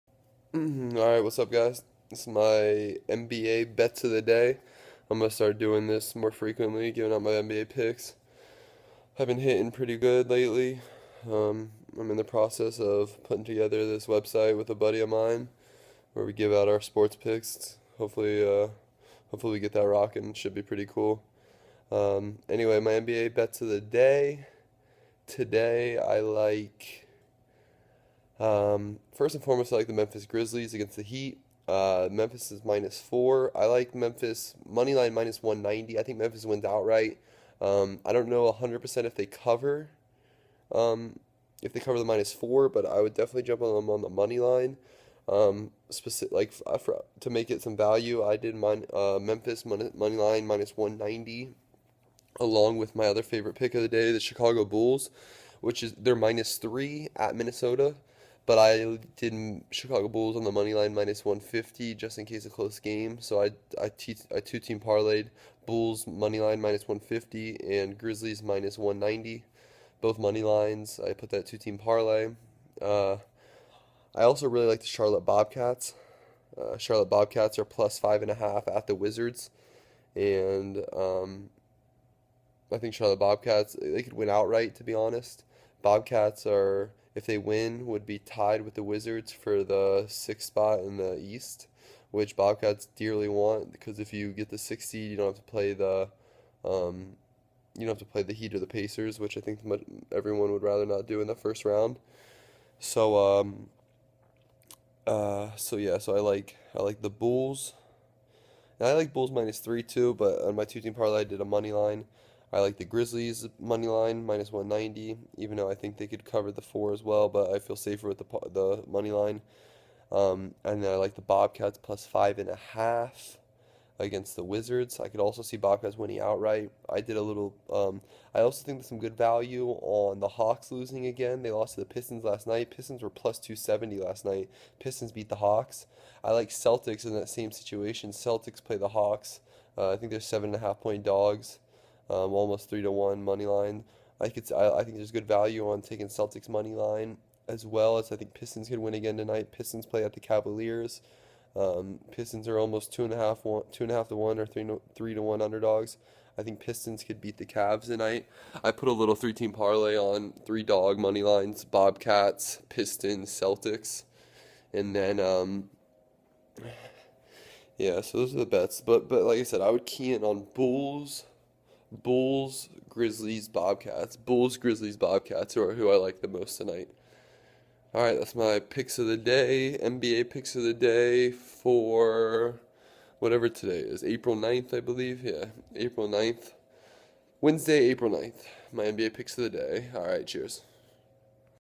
NBA Bets of the Day (April 9th, 2014)
I give out my NBA bets of the day for April 9th, 2014.